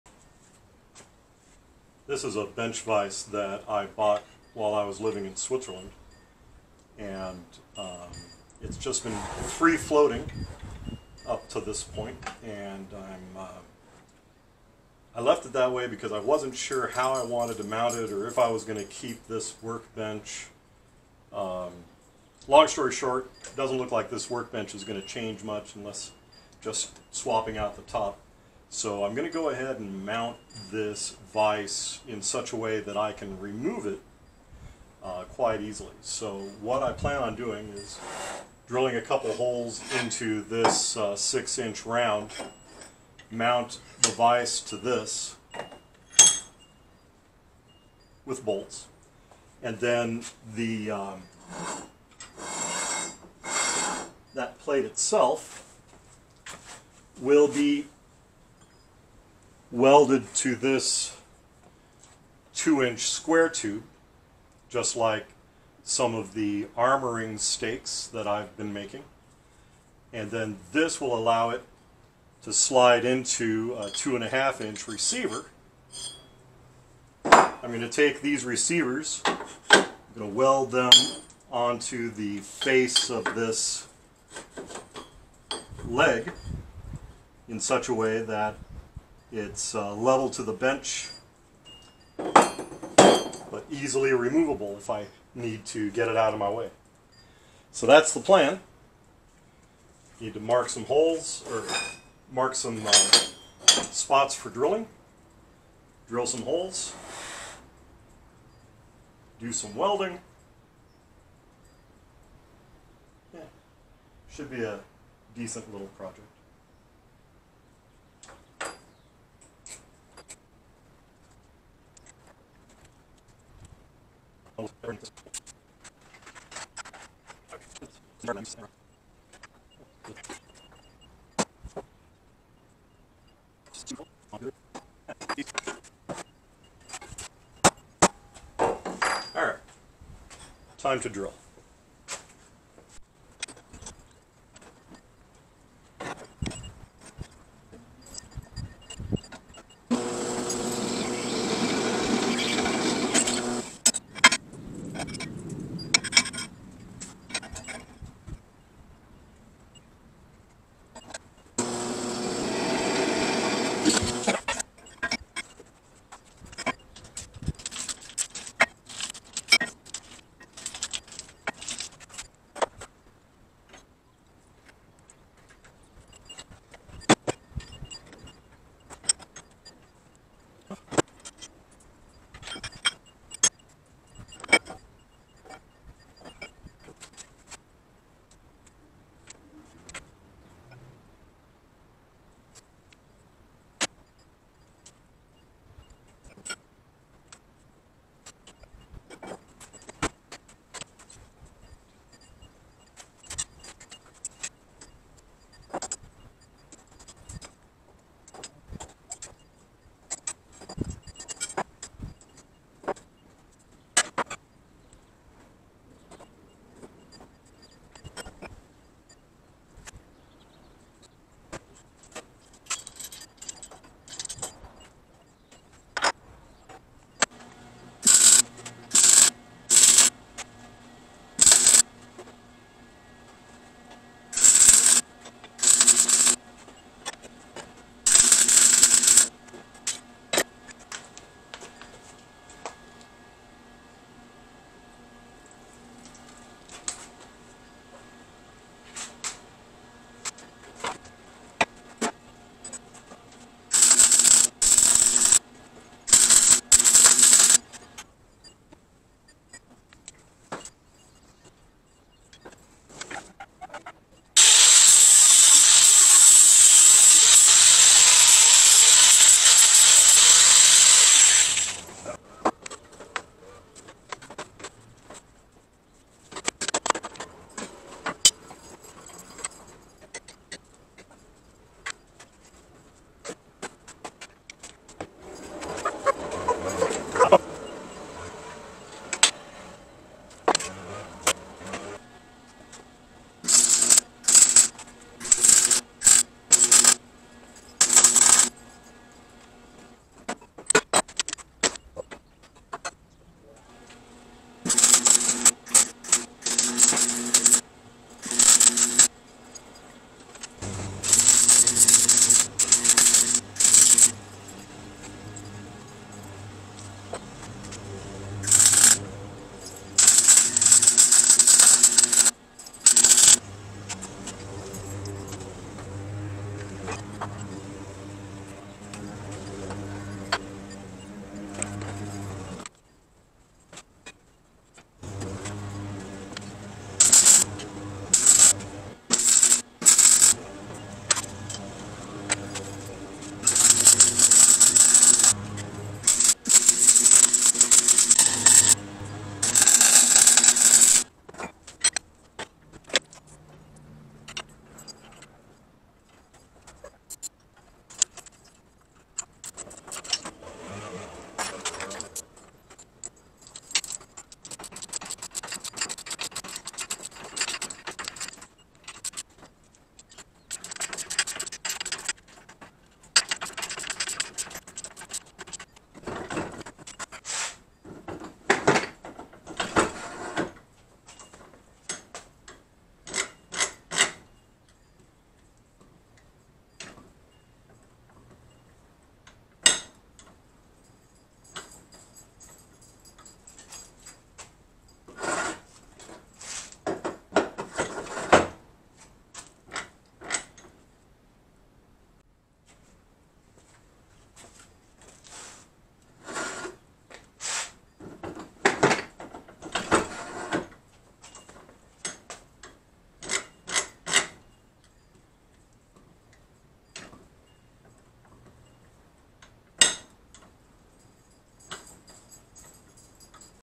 Adding a Bench Vise Mount to my Work Bench
Adding a Bench Vise Mount to my Work Bench which allows the vise to be removed, turned 90 degrees, or moved to my armoring stake cart (not pictured).